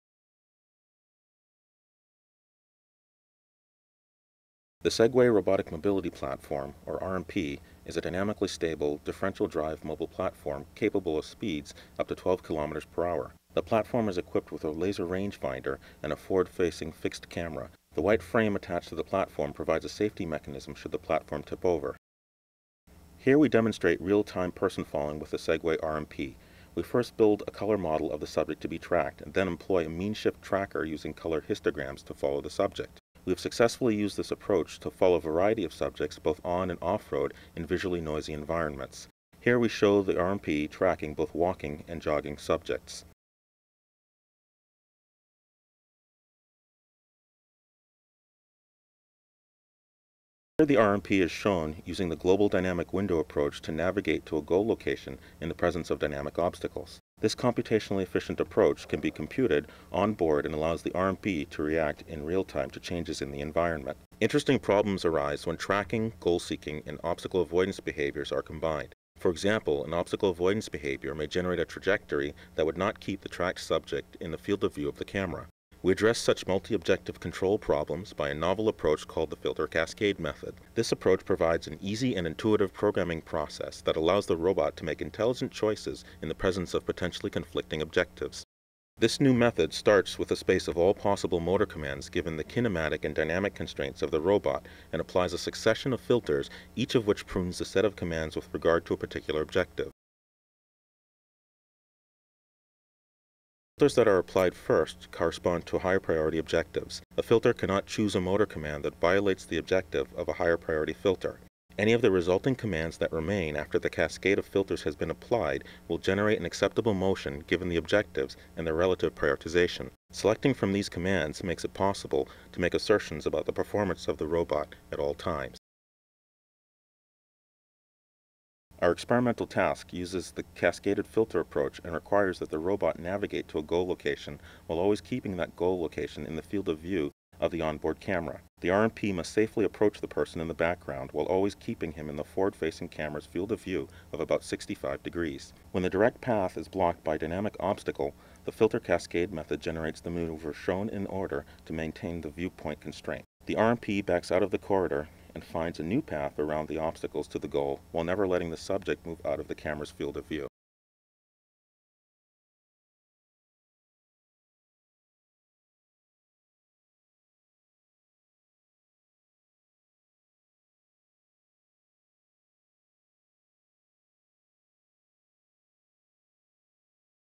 A Cascaded Filter Approach to Multi-Objective Control using the Segway RMP
Thibodeau, Bryan J., Stephen W. Hart, Deepak R. Karuppiah, John D. Sweeney, and Oliver Brock. A Cascaded Filter Approach to Multi-Objective Control using the Segway RMP. In Video Proceedings of the IEEE International Conference on Robotics and Automation, New Orleans, USA, April 2004.